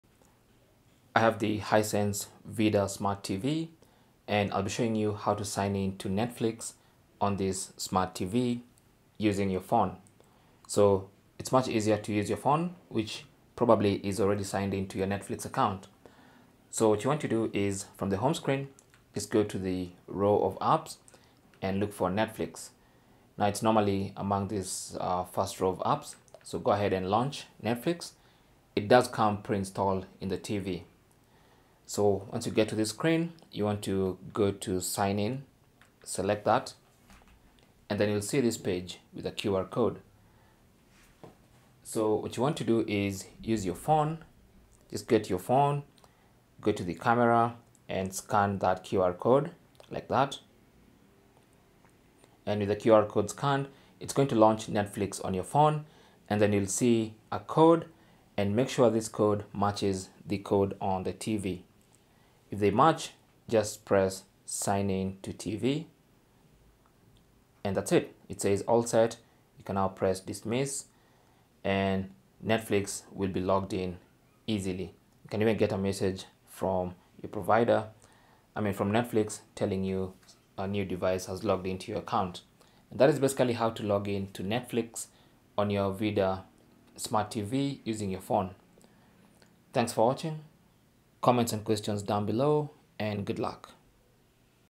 Hisense VIDAA Smart TV: How To Sign In To Netflix Using Phone
Our comprehensive guide on how to easily sign in to Netflix using your phone on a Hisense VIDAA Smart TV. In this step-by-step tutorial, we walk you through the simple process of linking your phone to your Hisense VIDAA TV to seamlessly access your Netflix account.

Whether you're new to the Hisense VIDAA platform or looking to connect Netflix through your mobile device, this tutorial provides clear instructions to make the setup hassle-free. Join us as we demonstrate the quick and convenient steps to pair your phone and navigate through the Netflix login process on your Hisense VIDAA Smart TV.

Sign In To Netflix Using Phone On Hisense VIDAA Smart TV:

Step 1: Go to the home screen of your Hisense VIDAA Smart TV. If you're not on the home page, press the home button on the remote control.

Step 2: Using your remote control, scroll down once to the apps section. Here, you'll see a list of all the pre-installed apps. Find and select the 'Netflix' app. It is typically the first one in the app's section. Netflix should be installed by default.

Step 3: Ensure that both your Hisense VIDAA Smart TV and smartphone are connected to the same Wi-Fi network.

Step 4: Open Netflix on your Hisense VIDAA Smart TV and click on "Sign In". From the resulting page, make sure you're on the "Use Phone" tab, with a number code and QR Code.

Step 5: Use the Camera app or QR Code Scanner on your smartphone to scan the Netflix QR code provided in the "Use Phone" tab.

Step 6: With the QR Code scanned, the Netflix app will launch on your Phone. Here, you will see a code provided. Ensure that the code on your TV matches the code shown on your phone screen. If it does, tap on "Sign In to TV".

Netflix will now be logged in easily on your TV. You will also receive an email from Netflix, to confirm the same.

Cell Phone Tripod Adapter